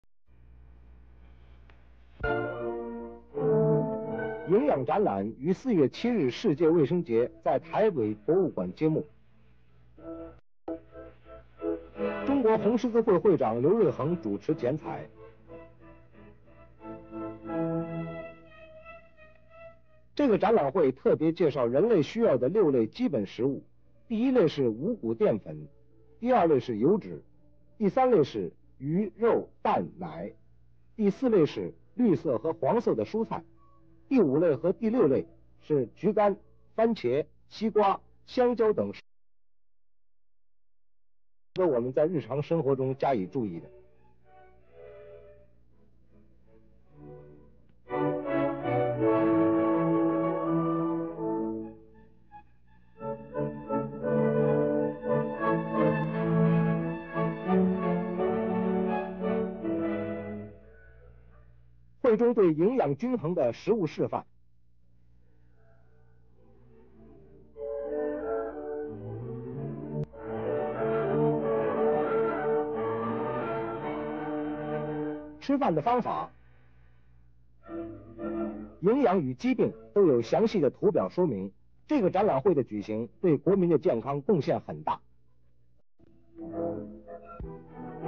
民國46年 營養展覽 1957, Nutrition Exhibition in Taipei, Taiwan
民國46年4月7日，世界衛生節暨營養展覽會在台灣省立博物館揭幕。中華民國紅十字會會長劉瑞恆主持剪綵，展覽會介紹人類需要的6類食物、各種營養元素、消化流程等。

作者：台灣電影文化公司。營養展覽。典藏者：國家電影及視聽文化中心。政府資料開放授權條款-第1版（Open Government Data License, version 1.0）。發佈於《臺灣影視聽數位博物館》

On April 7, 1957, the World Health Day and Nutrition Exhibition opened at the National Taiwan Museum in Taipei. The president of the Red Cross Society of the Republic of China, Liu Ruiheng, presided over the ribbon-cutting ceremony. The exhibition introduced the six types of food essential for humans, various nutritional elements, and the digestive process.

1957年 栄養展覧会

1957年4月7日、世界衛生デーと栄養展覧会が台北の台湾省立博物館（National Taiwan Museum | Taipei）で開幕しました。中華民国赤十字会の会長である劉瑞恒がリボンカットを行いました。展覧会では、人間に必要な6種類の食品、様々な栄養素、消化の過程などが紹介されました。

1957년 영양 전시회

1957년 4월 7일, 세계 보건의 날과 영양 전시회가 타이베이의 타이완성립 박물관(National Taiwan Museum | Taipei)에서 개막되었습니다. 중화민국 적십자회 회장 류루이헝이 리본 커팅을 주재했습니다. 전시회는 인간에게 필요한 6가지 식품, 다양한 영양소, 소화 과정 등을 소개했습니다.